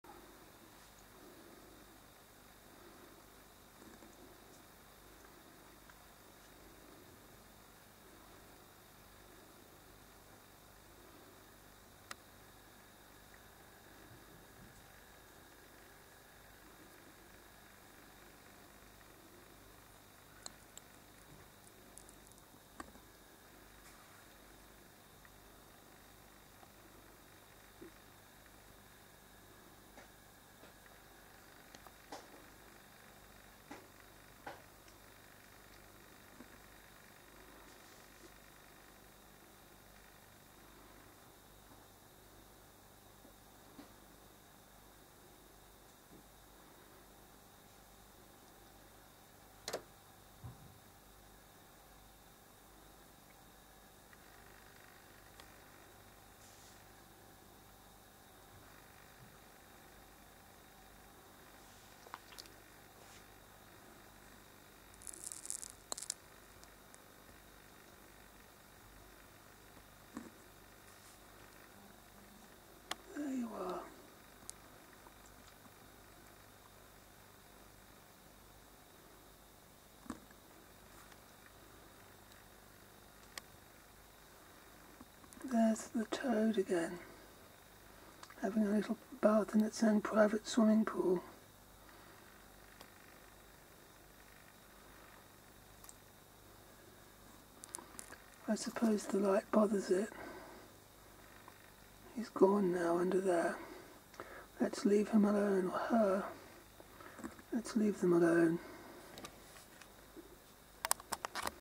Toad-in-a-Puddle ◕‿◔ ☂
That toad seems to be around every night, lately; so I thought he/she deserved a moment of glory on YouTube again. If you know the species of toad/frog, then please let me know. Very cute, anyhow.